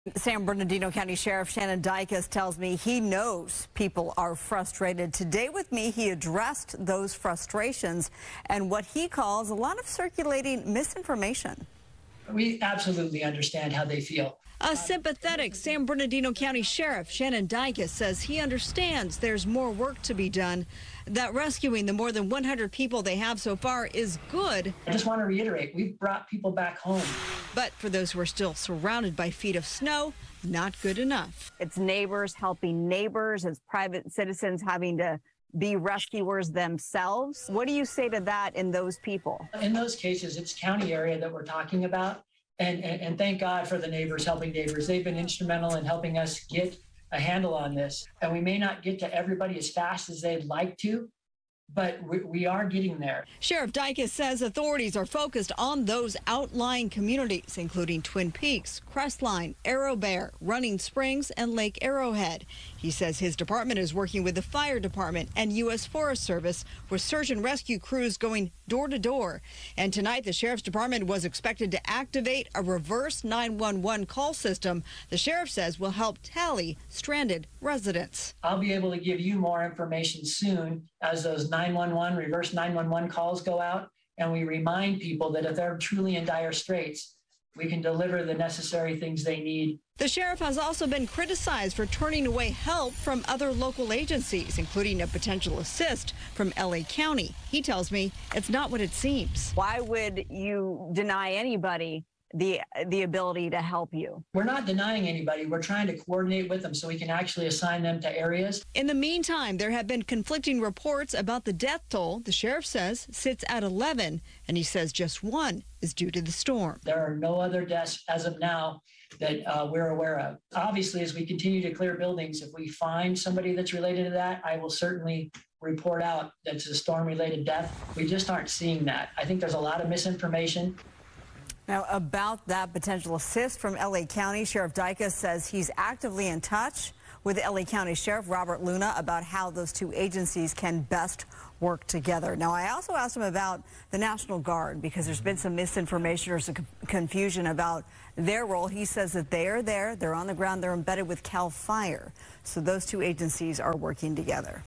San Bernardino Co. Sheriff responds to mountain rescue criticism
San Bernardino County Sheriff Shannon Dicus says he knows people trapped in the mountains are frustrated, while clarifying what he called "misinformation" on the rescue efforts.